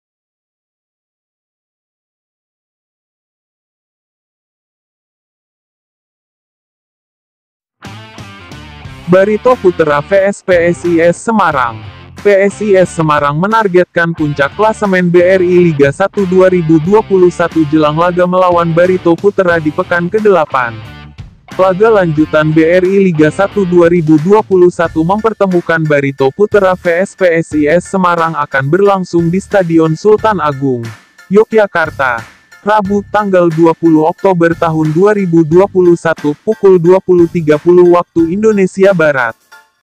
PS BARITO PUTERA VS PSIS SEMARANG LIVE PADA PEKAN KE-7 BRI LIGA 1 INDONESIA 2021 DI STADION SULTAN
Laga lanjutan BRI Liga 1 2021 mempertemukan Barito Putera vs PSIS Semarang akan berlangsung di Stadion Sultan Agung, Yogyakarta, Rabu (20/10/2021) pukul 20.30 WIB.